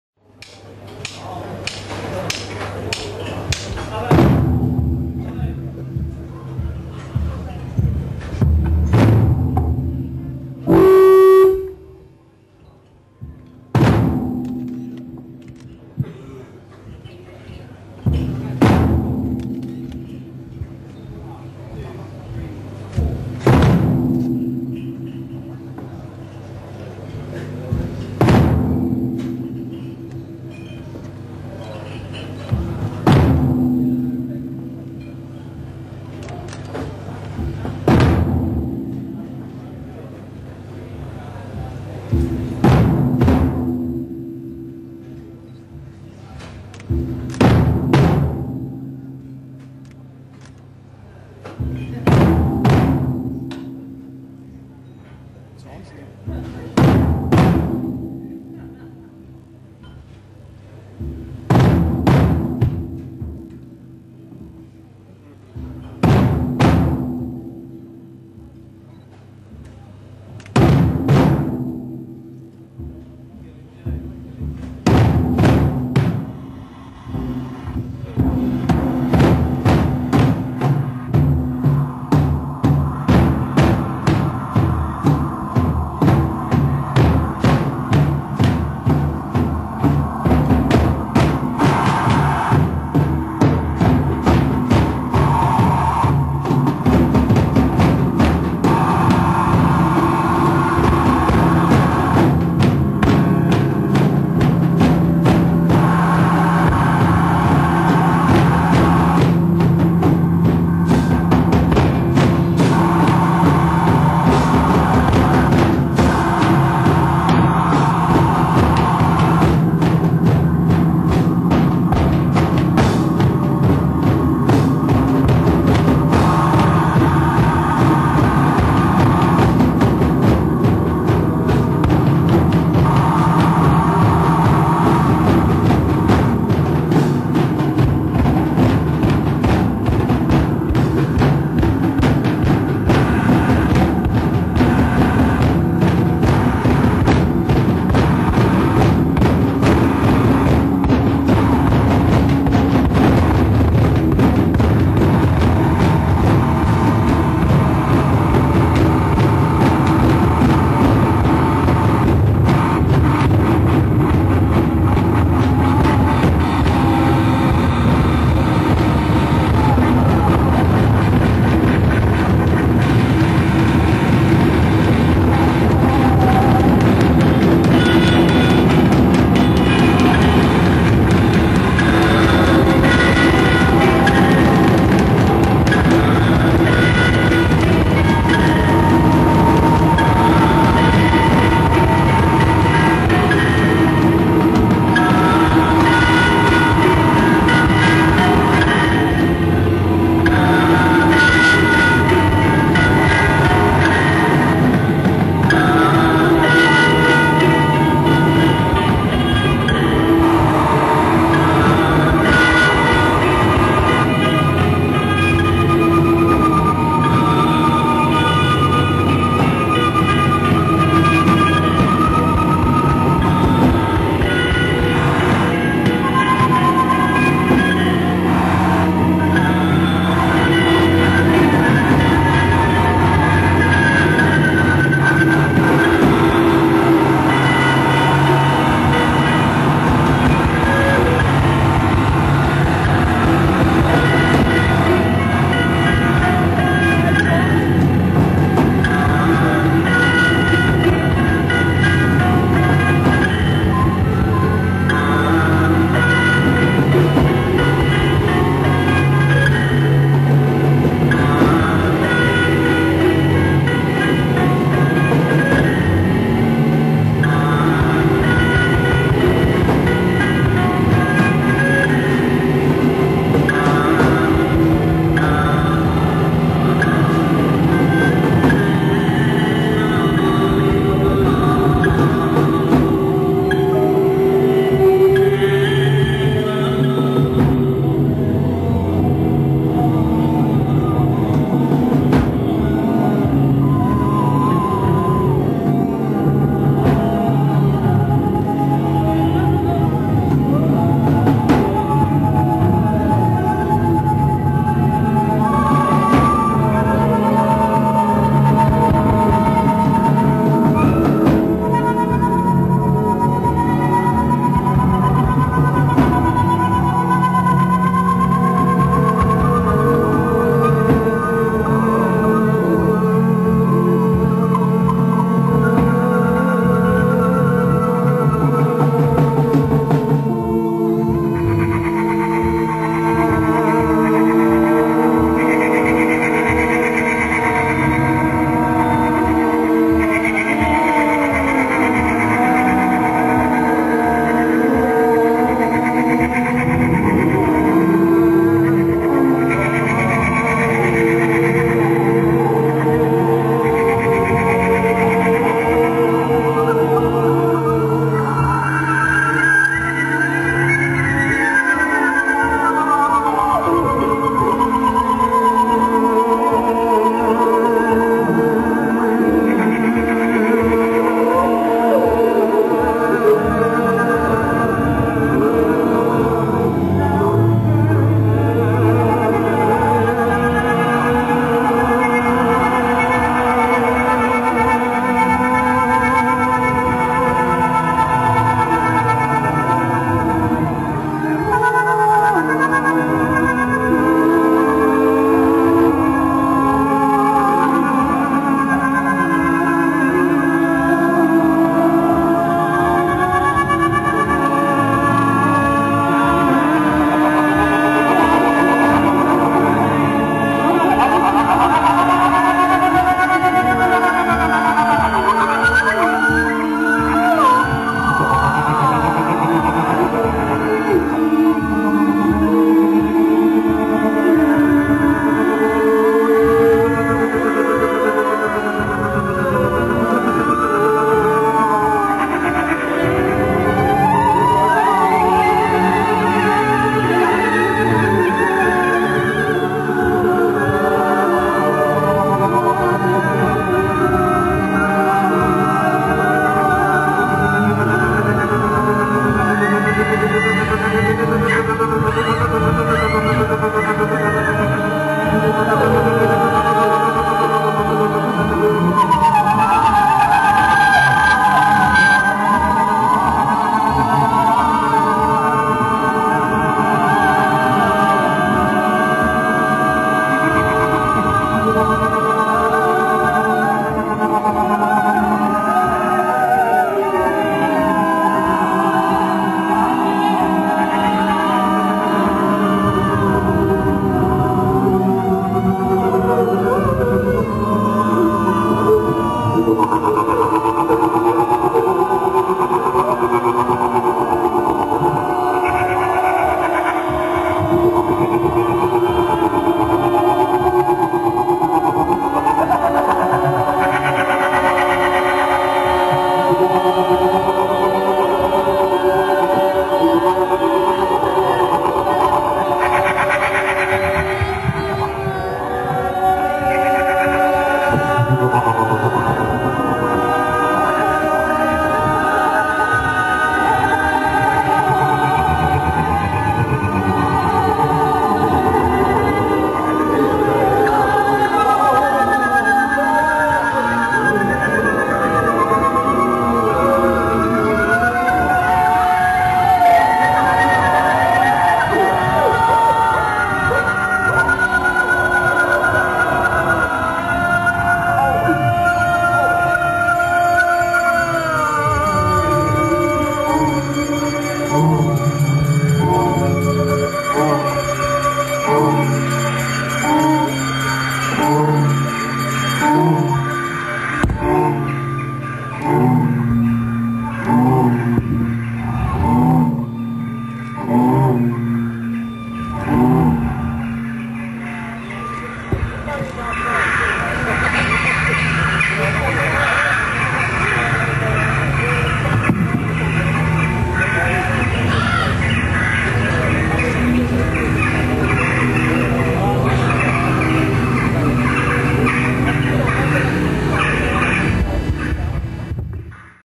Leopard Leg @ London, Hippodrome
I recorded this on friday the 13th 2006 in Uk/London/Hippodrome on the FRIEZE music festival.
check out these nice lads, first 10 minutes from the set